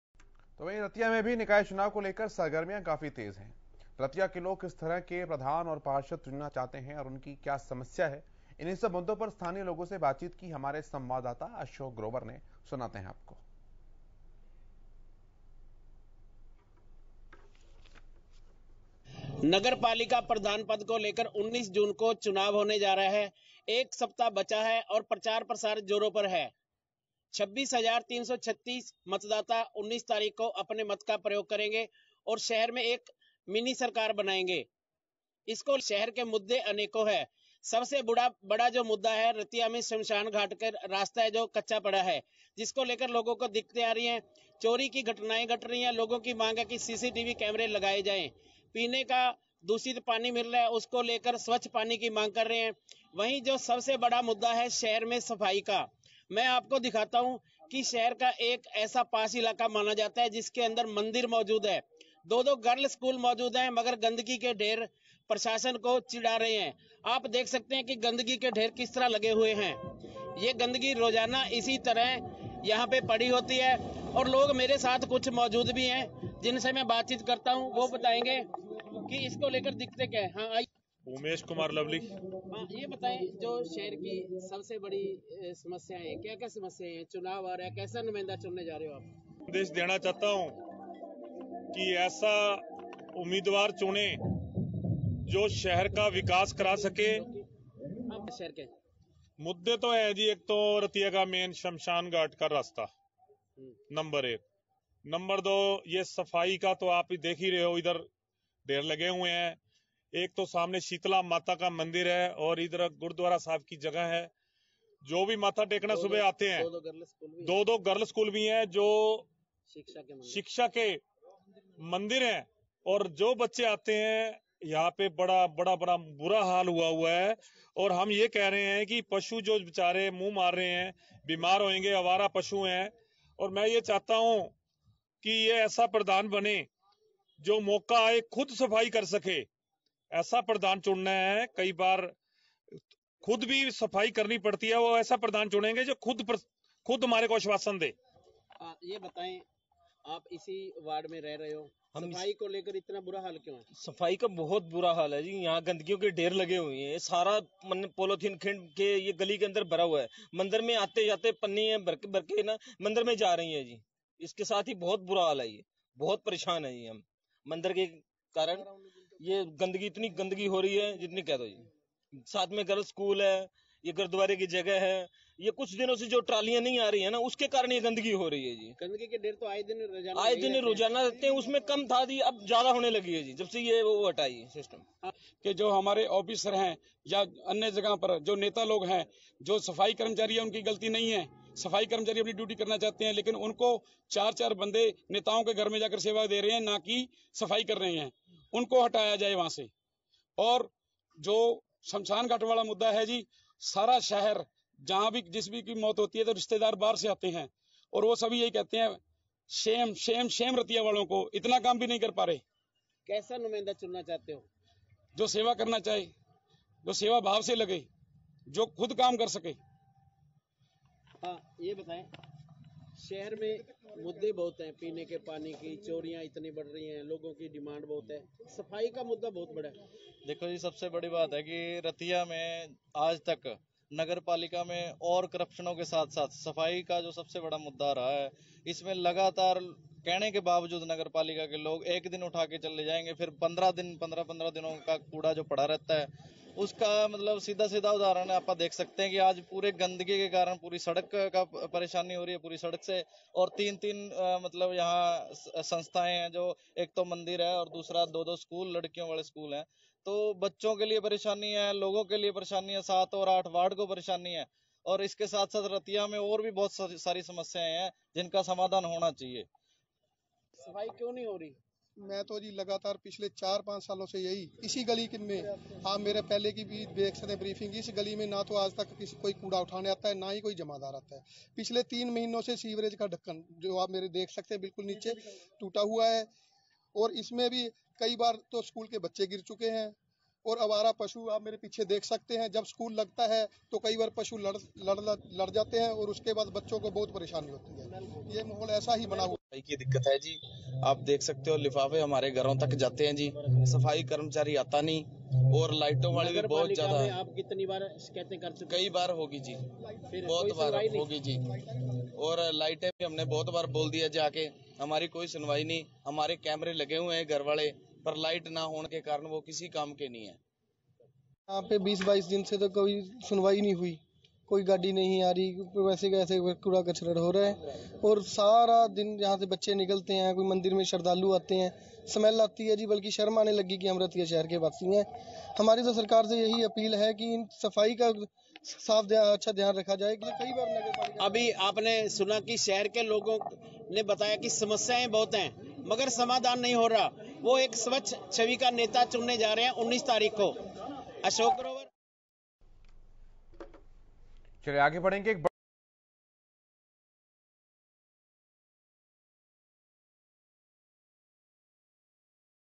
रतिया में निकाय चुनाव की तैयारी पूरी, स्थानीय लोगों ने बताई अपनी समस्याएं
रतिया में भी निकाय चुनाव को लेकर सरगर्मियां तेज हो गई है. रतिया के लोग किस तरह के प्रधान और पार्षद चुनाना चाहते हैं और उनकी क्या समस्या हैं.